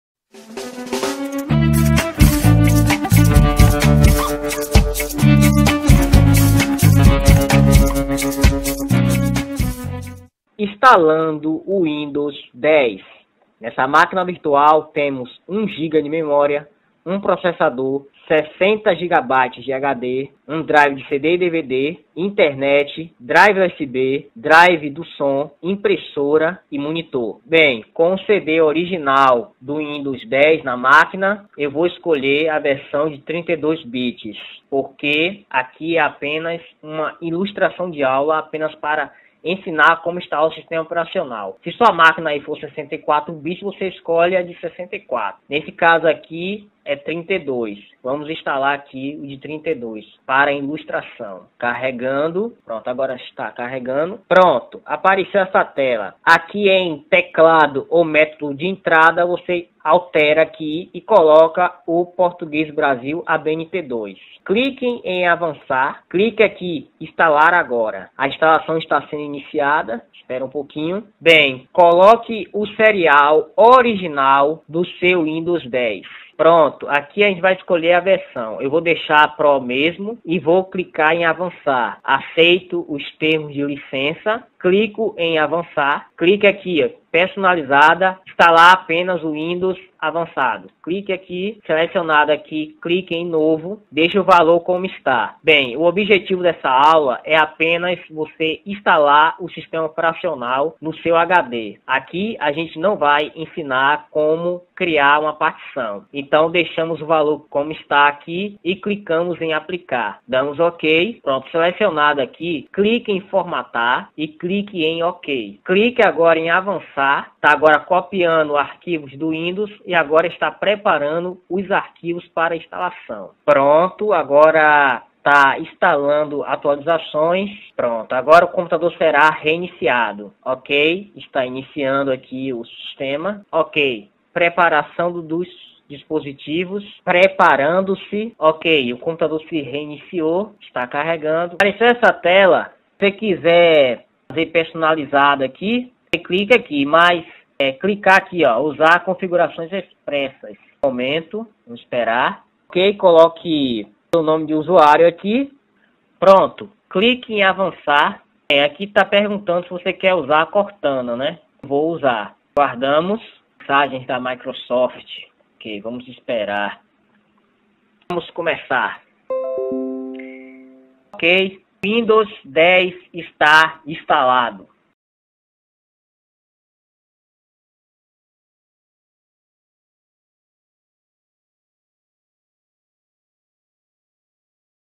🔴 Guia completo para instalar o Windows 10 passo a passo | Formatação e instalação simplificadas
★ INFORMAÇÕES IMPORTANTES ★
Descubra como instalar o Windows 10 de maneira fácil e rápida! Este guia completo apresenta o processo de formatação e instalação do Windows 10, com instruções passo a passo.

Sua contribuição é fundamental para o crescimento do canal. Aproveite o vídeo em alta definição (1080p60 HD)!

Vídeo desenvolvido por Albert Vinícius Maia.